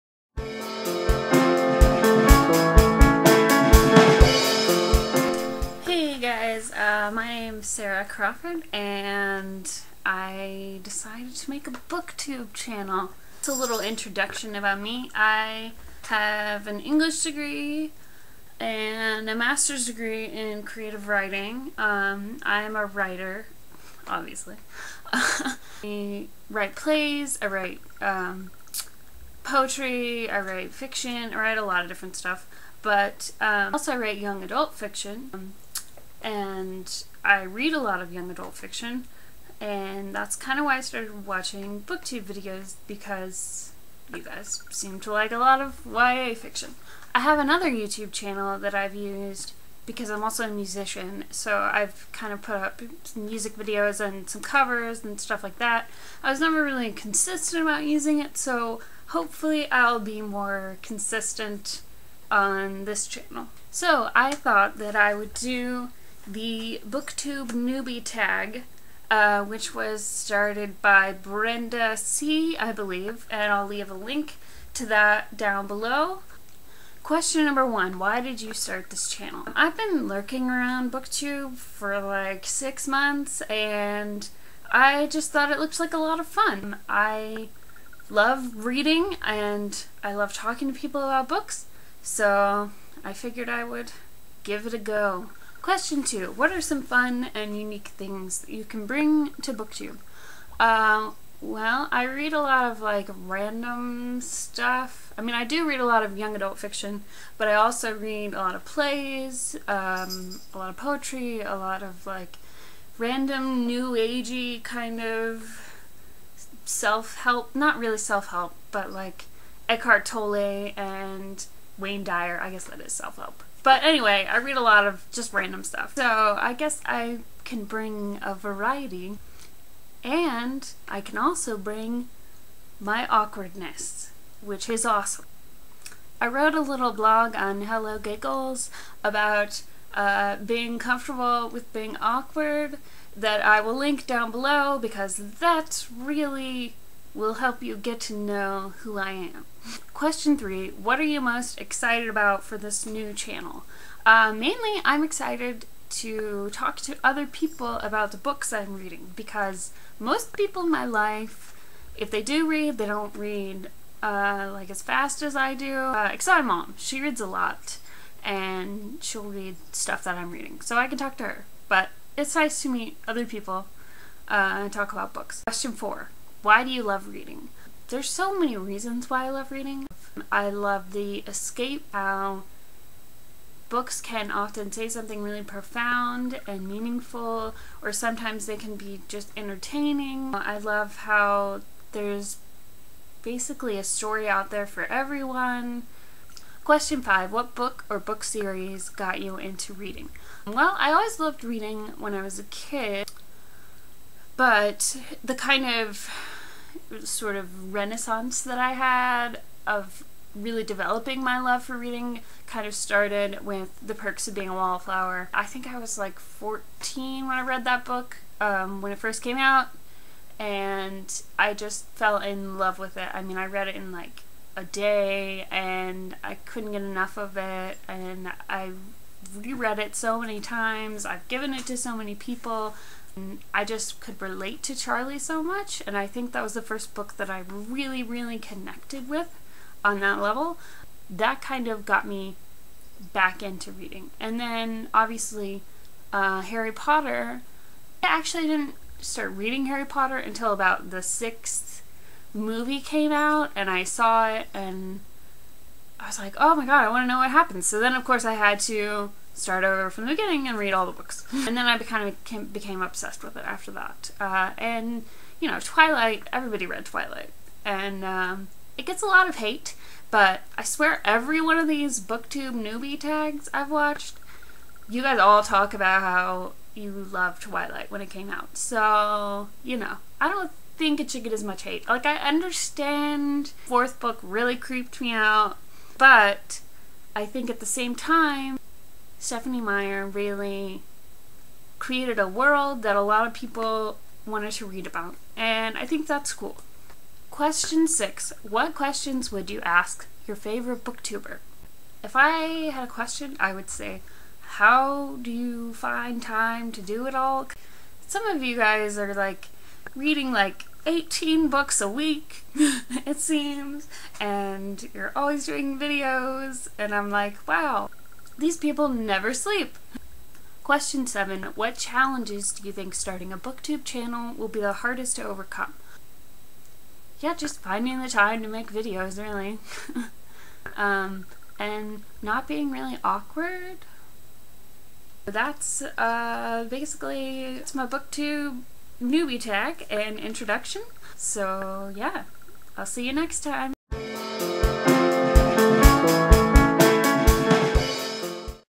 Booktube Introduction and Newbie Tag
(Next time I won't A. leave my sock drawer open in the background B. say "ummm," "you know," and "like" so much and C. do really choppy edits. I will try to bring up the volume, too. Oh and the sync got a little off there in the middle, but hey! It was my first booktube video so you know!)